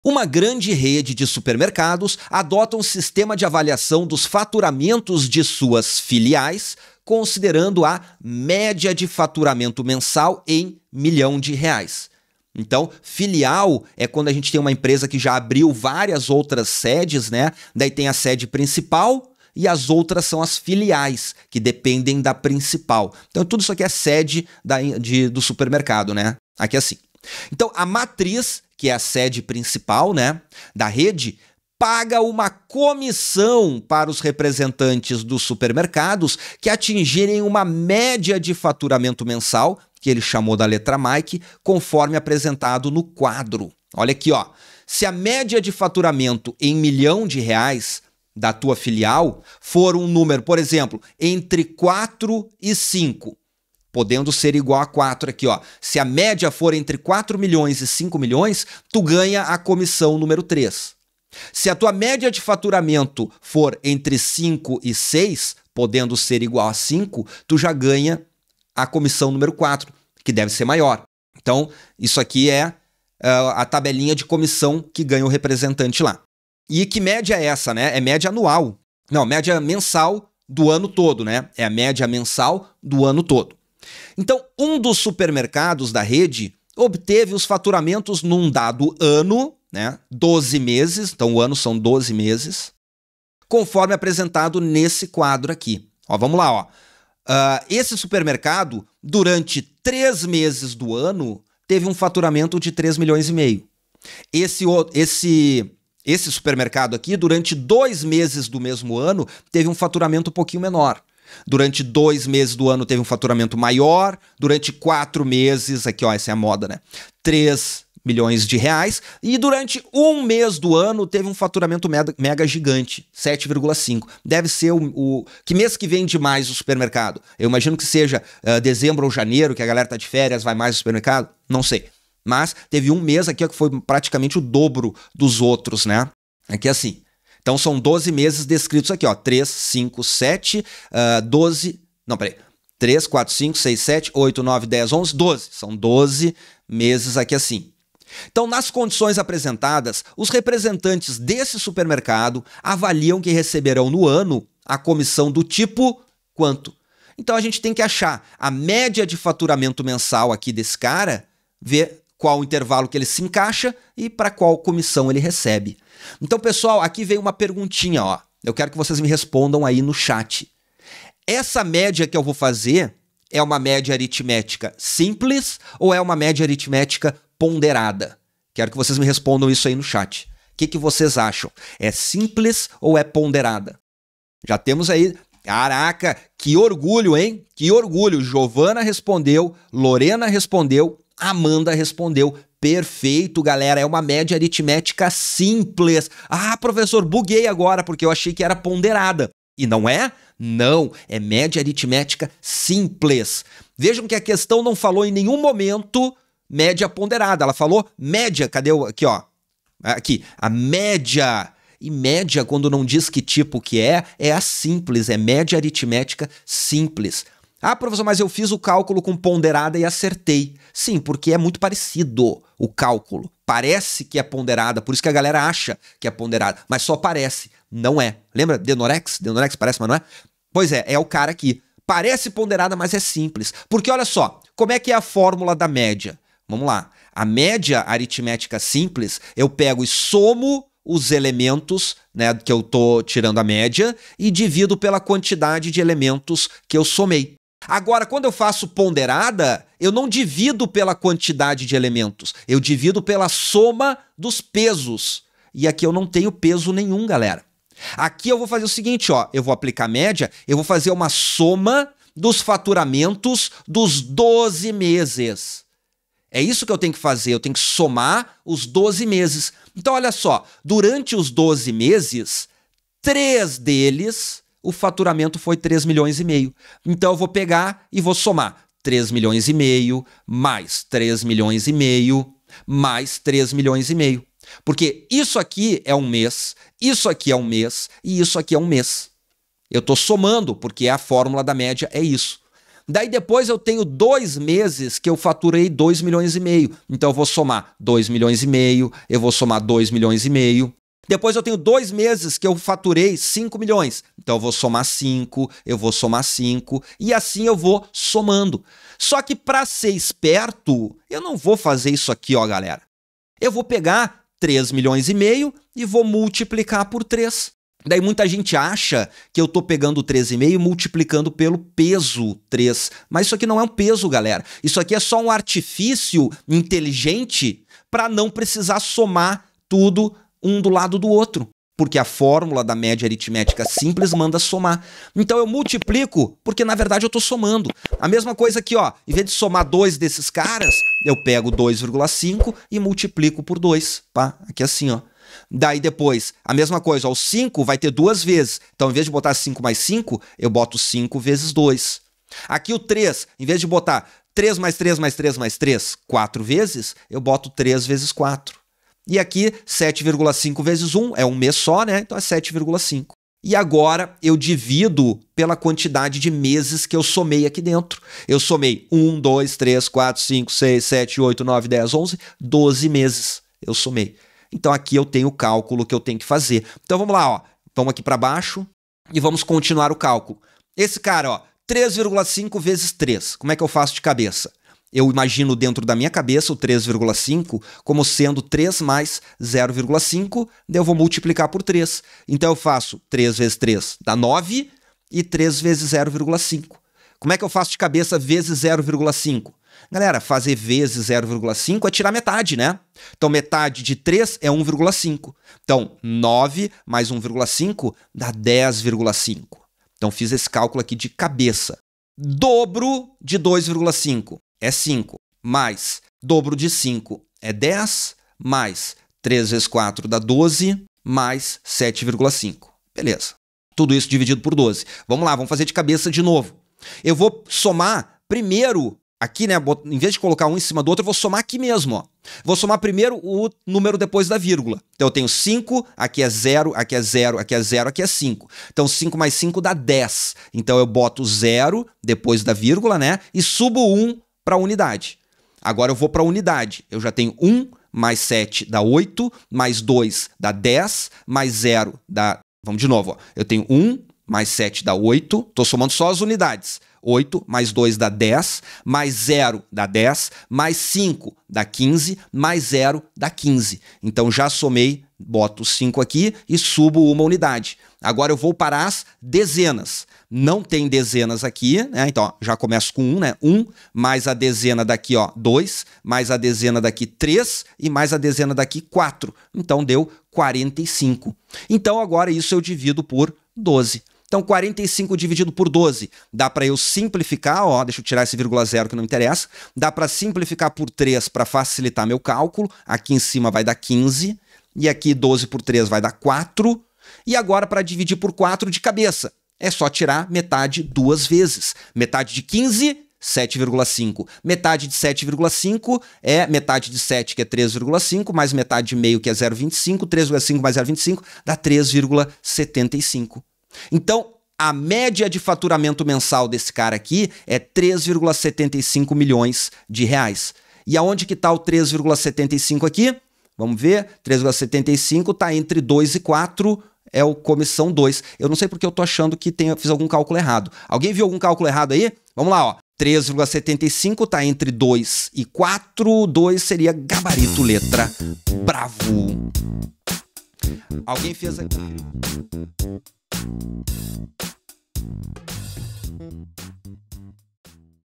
[ENEM 2021] 140 📘 MÉDIA ARITMÉTICA Uma grande rede de supermercados adota um sistema de avaliação
[140] Uma grande rede de supermercados adota um sistema de avaliação dos faturamentos de suas filiais, considerando a média de faturamento mensal em milhão. A matriz da rede paga uma comissão para os representantes dos supermercados que atingirem uma média de faturamento mensal (M), conforme apresentado no quadro.
Um supermercado da rede obteve os faturamentos num dado ano, conforme apresentado no quadro.
Nas condições apresentadas, os representantes desse supermercado avaliam que receberão, no ano seguinte, a comissão de tipo

ENEM 2021 - 2ª Aplicação - Prova de Matemática Resolvida pelo Prof. Caju.

Problema sobre Média Aritmética

Questão 140 da Prova Azul - ENEM 2021

Quer contribuir para manter meu trabalho de compartilhar conhecimento pra todo Brasil?